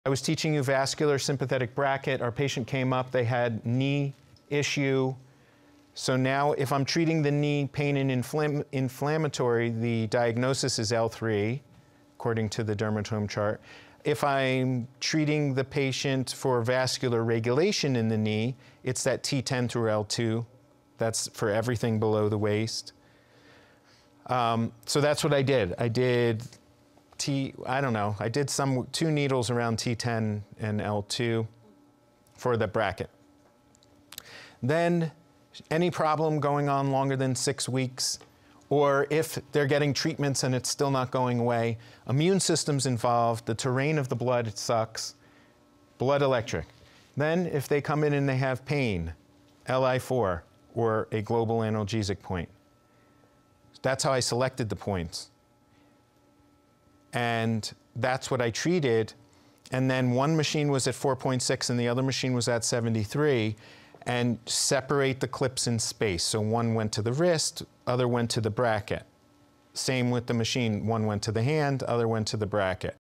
Unlock Knee Pain Relief Expert Explains Surprising Treatment Methods
Most acupuncturists overlook this vascular pathway. Dr. Jeremy Steiner MD, PhD explains how strategic needle placement and specific machine frequencies can reduce knee pain and inflammation.

Learn how electroacupuncture addresses the root immune system imbalances in chronic conditions for lasting results.

Watch the full training at electroacupunctureinstitute.com